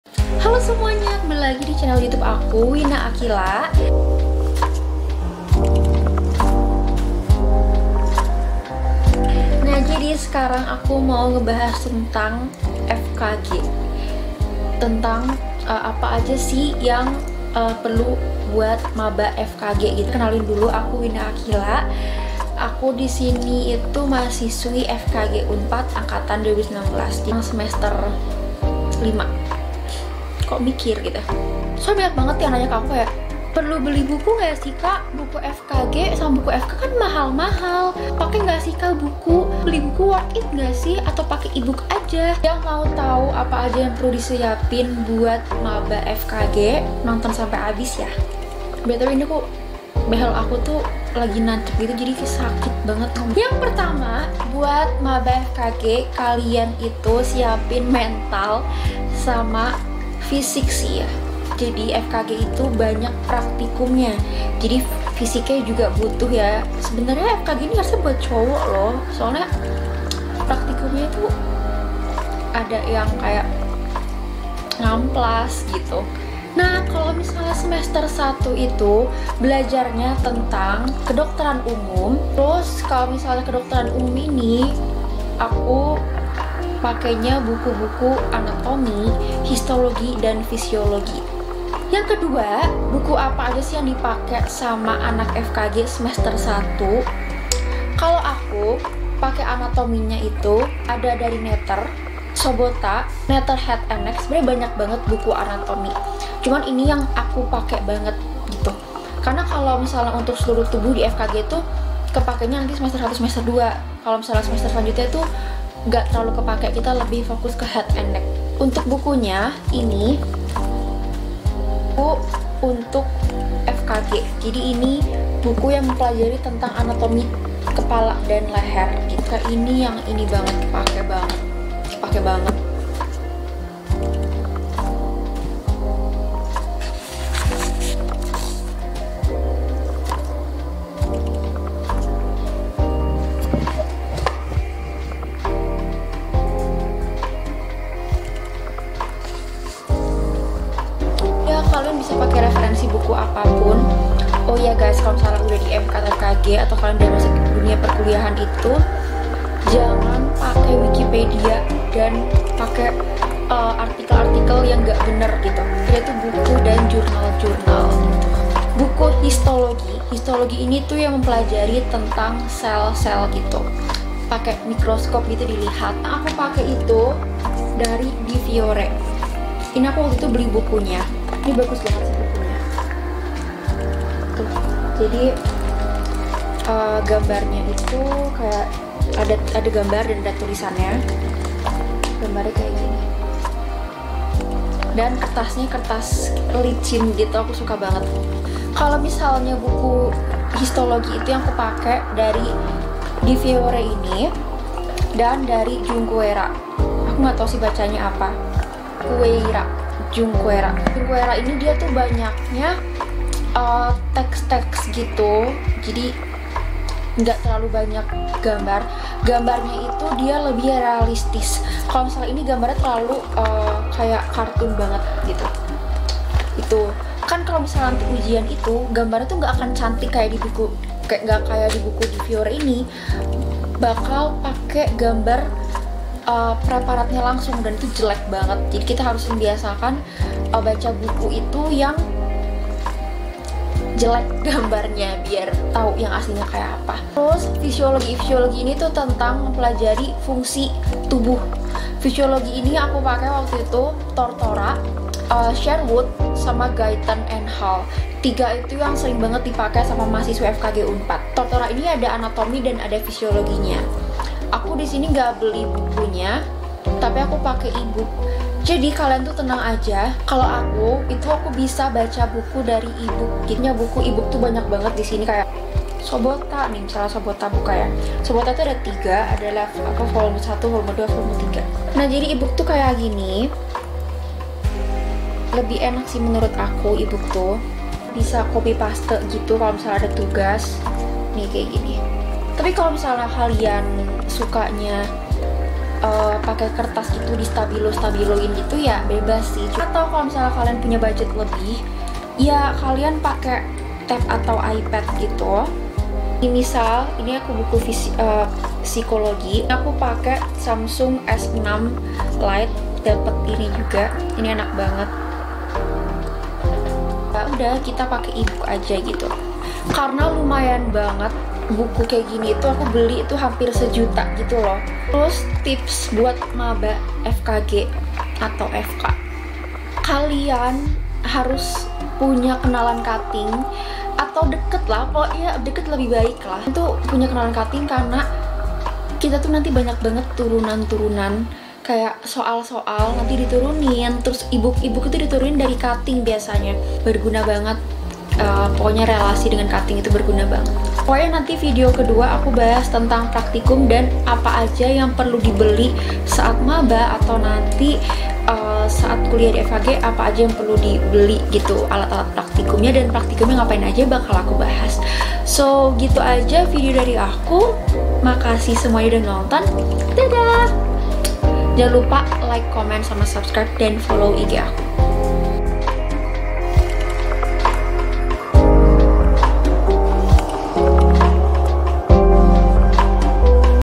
TIPS & BUKU BUAT UNTUK MABA FKG
tips buat mahasiswa baru fkg dan buku apa aja yang waktu itu aku pake pas semester 1
halo semuanya semoga video aku membantu ya!
sebenernya banyak bgt tips tapi aku mau buay video terpisah aja
yang mau aku buat video lain ttg college life bisa comment atau dm aku yaa

anatomi :
netter
sobotta
netter head n neck

histologi :
difiore
junqueira

fisiologi :
tortora
sherwood
guyton and hall